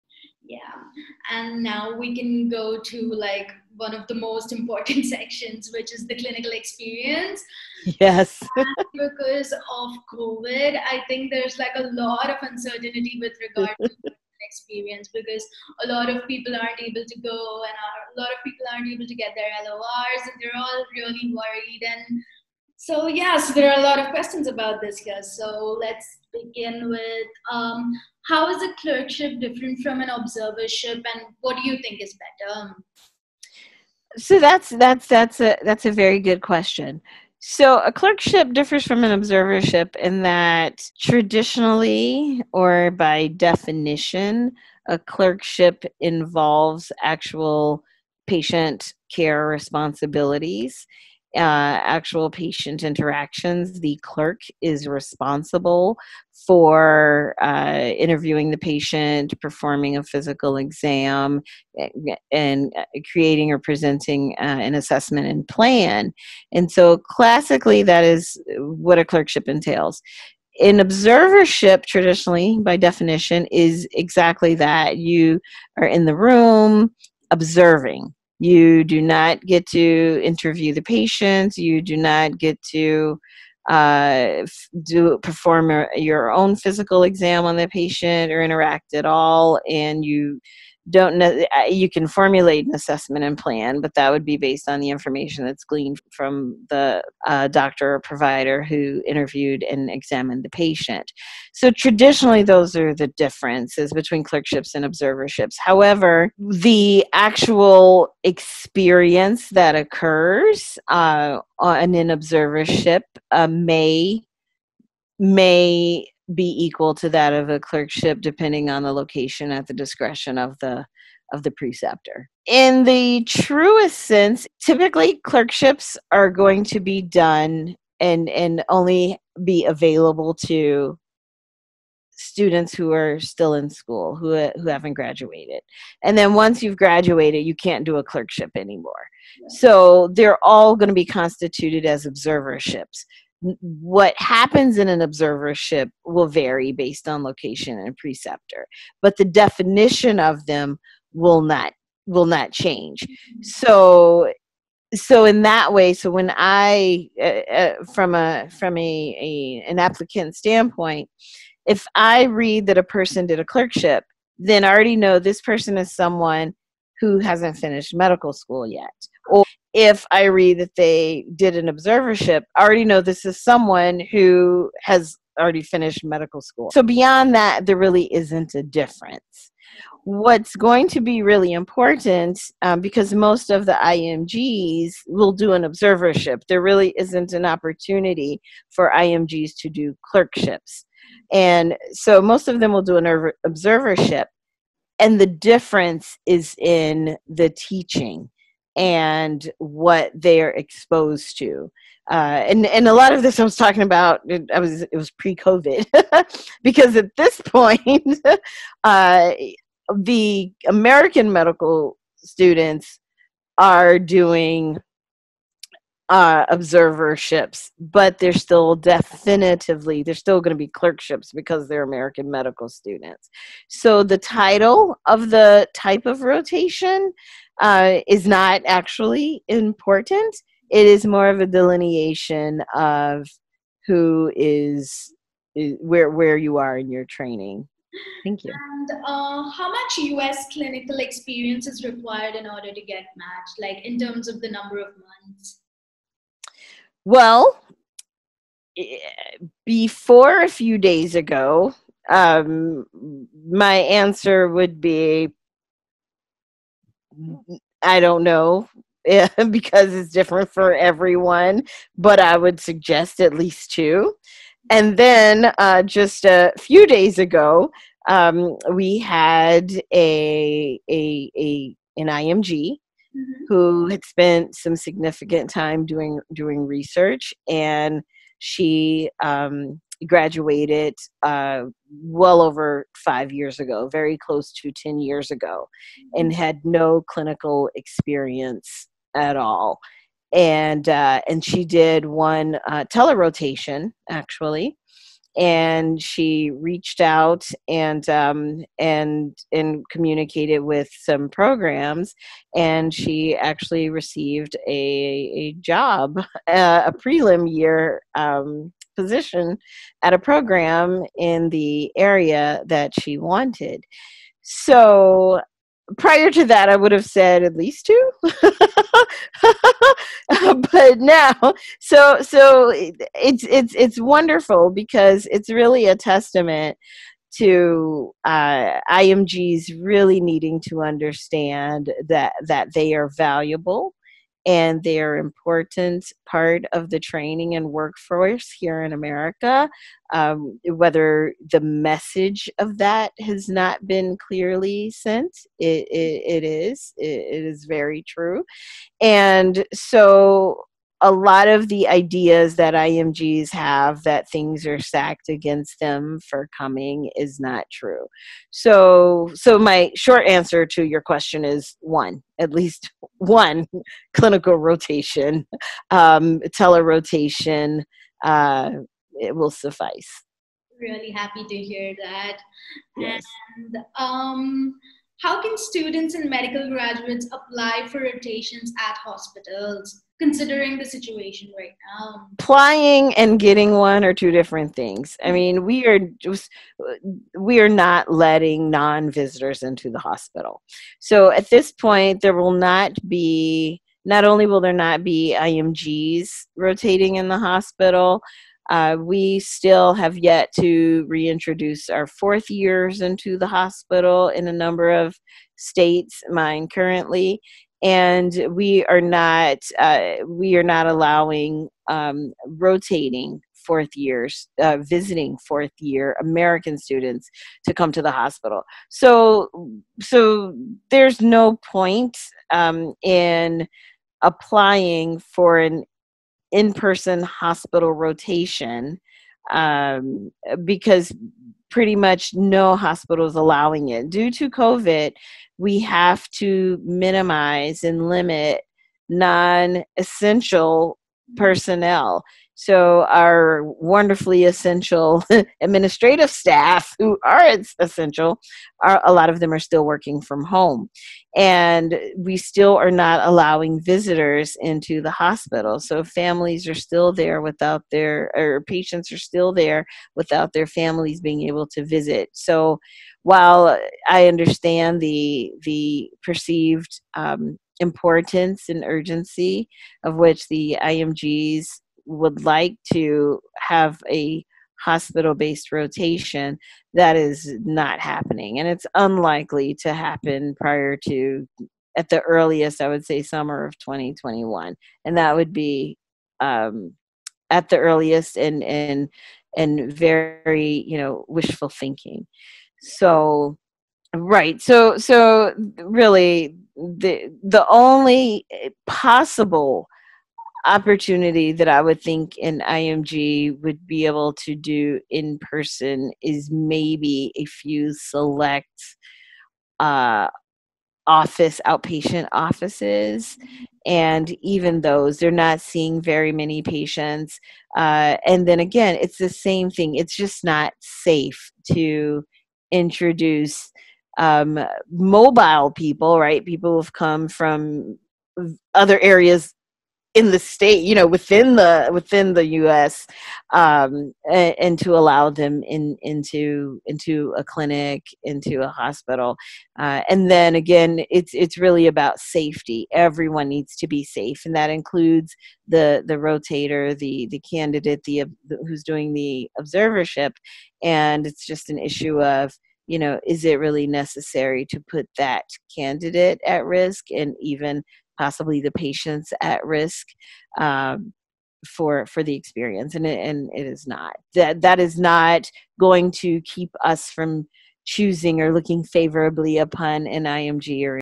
Q/A with a Program Director- US Clinical Electives/Observerships in 2020 for IMGs [Part 6/8]
Hi everyone!
I did a Q/A with Dr. Watts a few weeks back.
Dr. Tabitha Watts is a Pediatrics Emergency Medicine Attending at John H. Stroger, Jr. Hospital of Cook County

Since a lot of you might not have time to watch it, I split it into 8 parts based on the topic.

This is the sixth part in this series
We have discussed:

-Clerkship vs Observership. Which one is better?
Who is eligible for clerkship and who is eligible for a clerkship?
- What are American med students doing now?
- How much US clinical experience is required in order to get matched?
- How can students and graduates apply to hospitals for rotations considering the situation now?

👩‍⚕️ I'm a 5th year medical student, your study partner (:
👩‍💻 I make videos to guide students preparing for the USMLE and share my med experience here on my Youtube channel